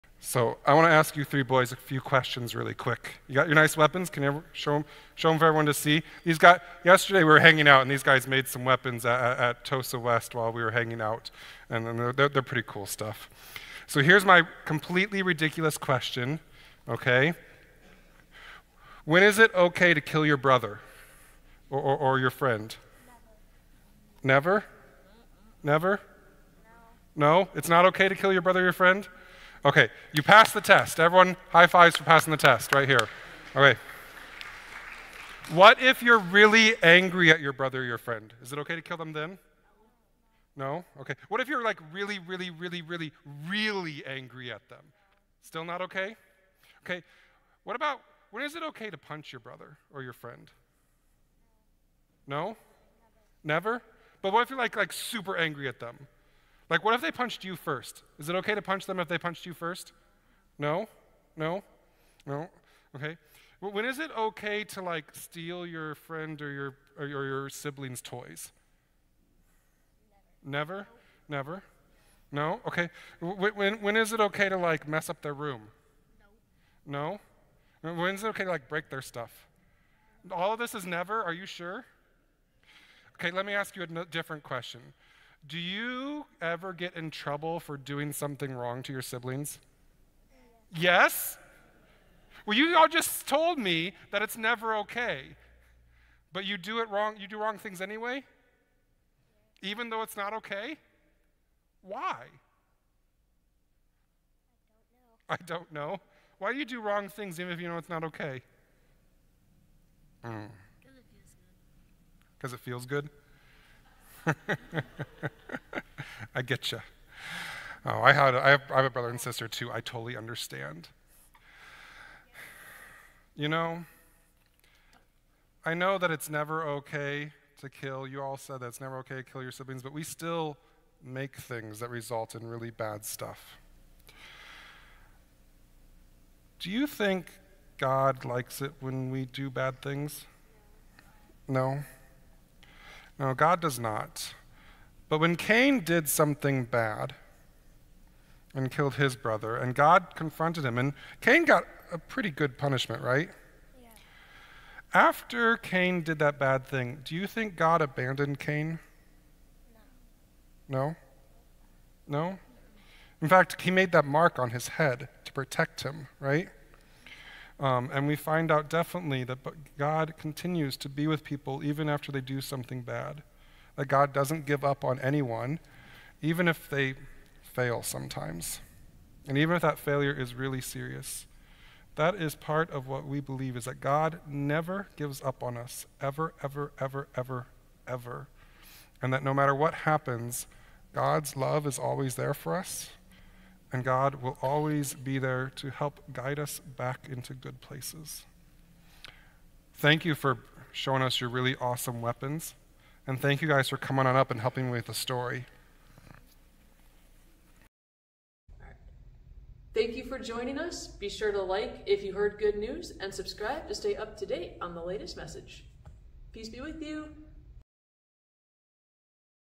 When is it OK to hurt someone?
Sermon for June 16, 2024
Based on Genesis 4
Summer Series on Genesis